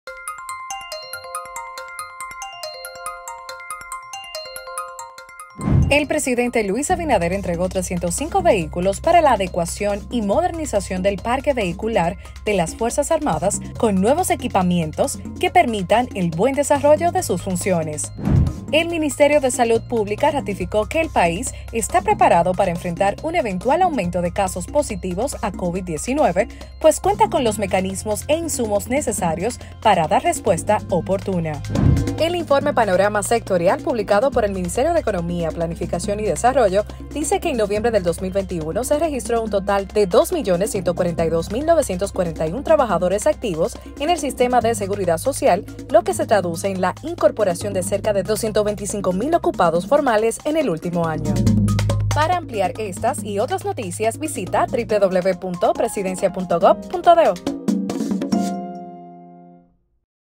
Infórmate En 60 Segundos 5/1/2022
Infórmateen60segundos: El Presidente @LuisAbinader entregó 305 vehículos para la adecuación del parque vehicular de las Fuerzas Armadas. @SaludPublicaRD ratificó que el país está preparado para enfrentar un aumento de casos COVID-19.